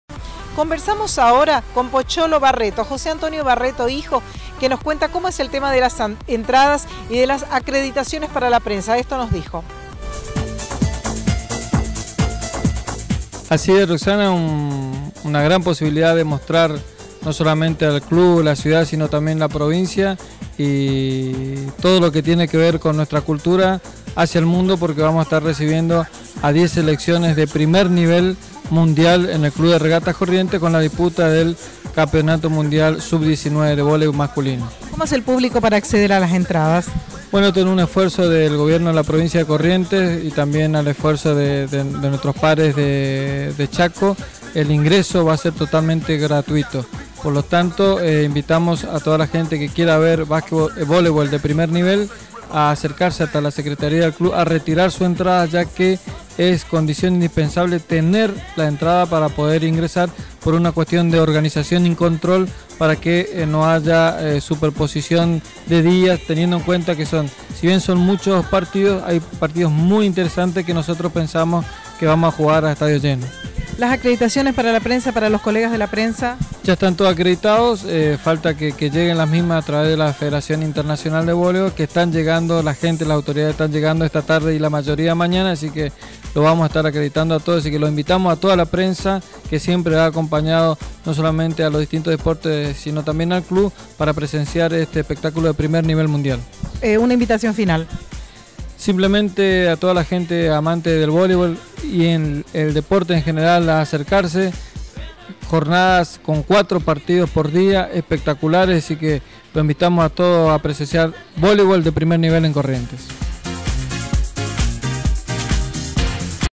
Club Regatas Corrientes 2da Parte
Ahora dialogamos con Pocholo Barreto . . .  Prensa del Club . . .